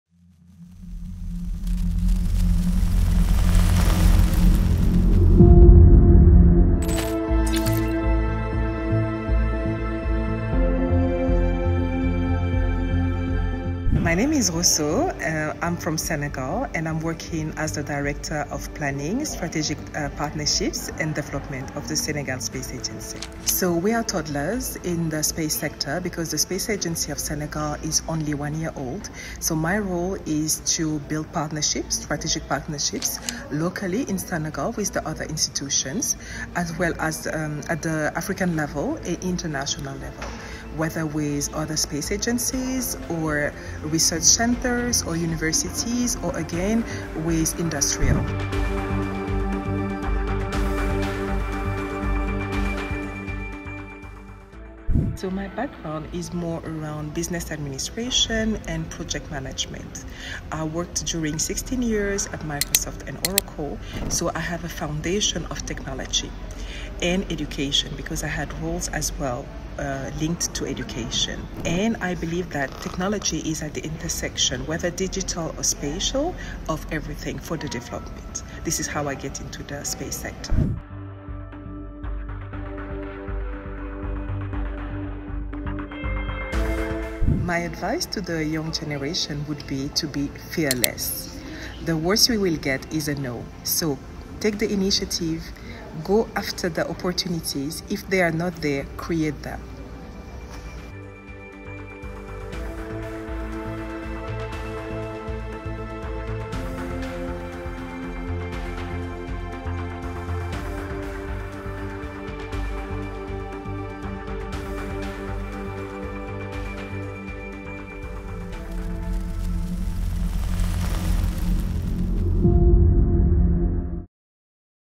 Building Africa's Space Future | Rosso on Space Tech for Communities | Friends of SpaceVrse | EP 29
🚀 Welcome to Friends of SpaceVrse!

In this inspiring episode, we sit down with Rosso, a  pioneering leader in digital and space technologies, transforming Africa's space landscape from Senegal. As the Director of Planning, Partnership, and Development at the Senegal Space Agency, Rosso is at the forefront of building strategic partnerships and fostering innovation across the continent.

Rosso's work demonstrates how space technology can be a powerful tool for social impact, particularly in providing access to technology and resources for communities that need it most. Her focus on empowering young girls in STEM creates ripple effects that will shape Africa's space future for generations to come.